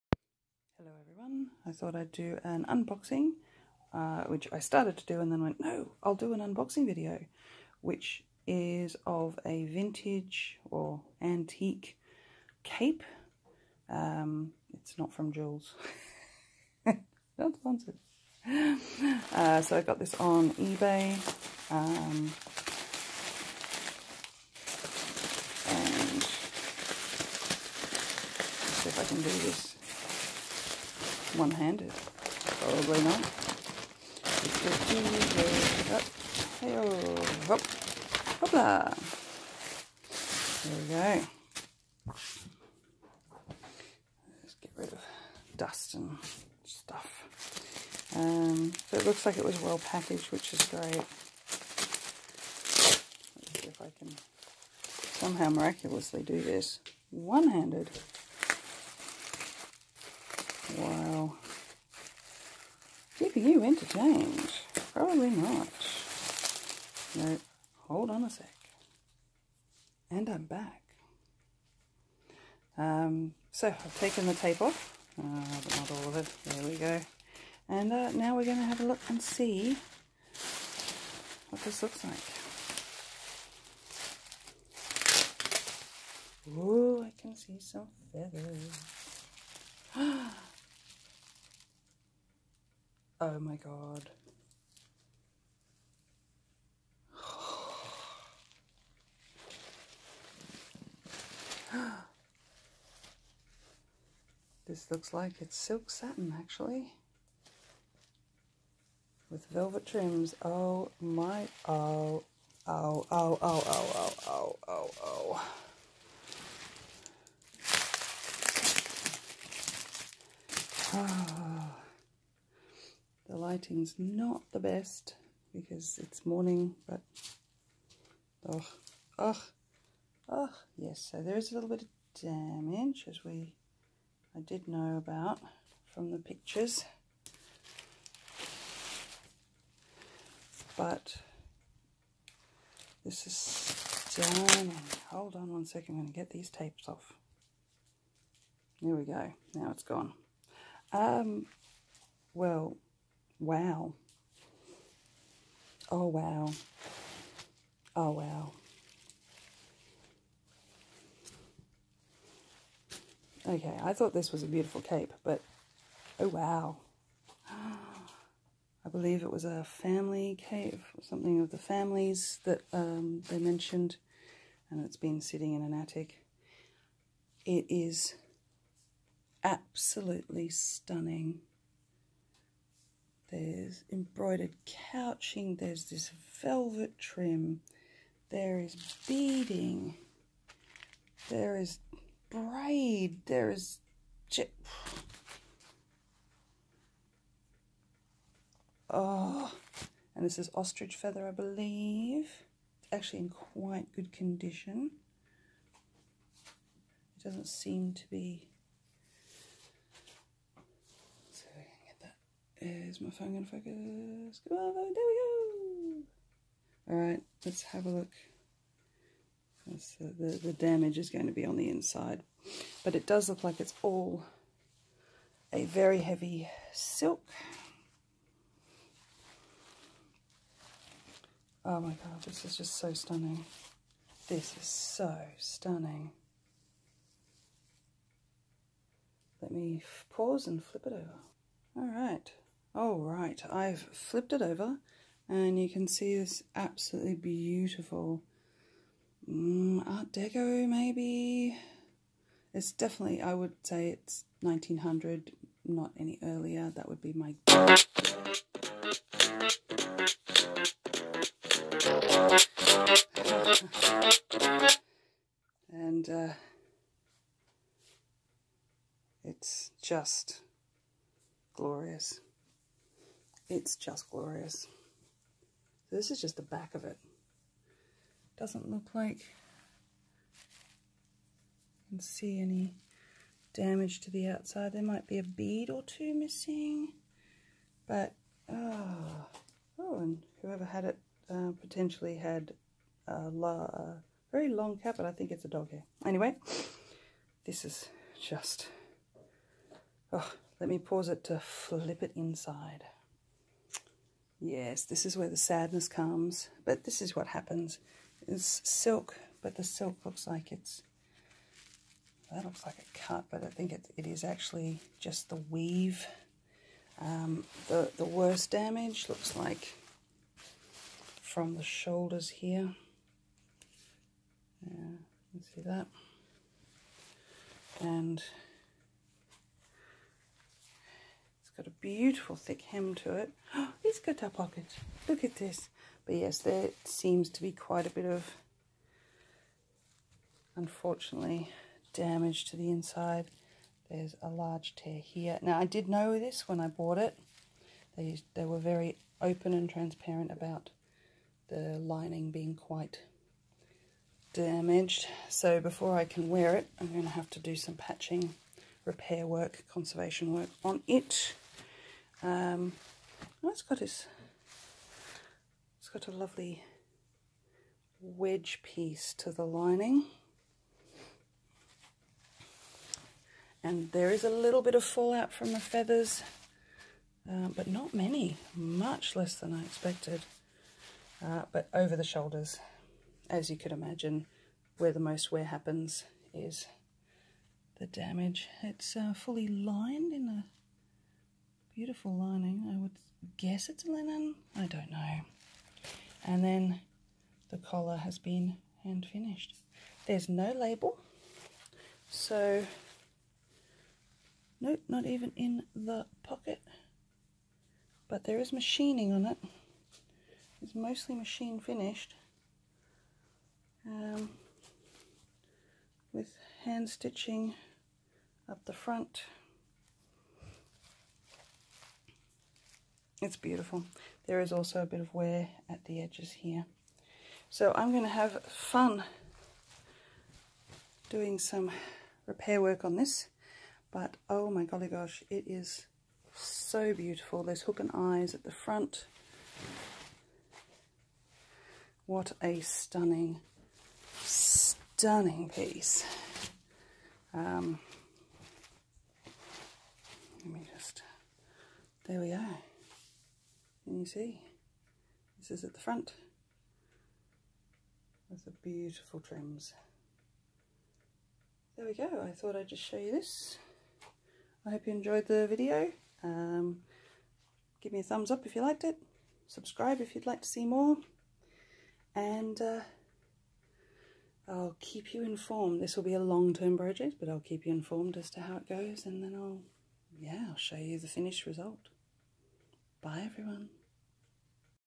Unboxing of antique cloak. Original silk cloak that is Soo stunning
I bought this antique clock on Ebay and I believe it's c1900.
It needs conservation which I will be looking into.
You get to see me unbox it and share my 1st thoughts and reactions to the garment.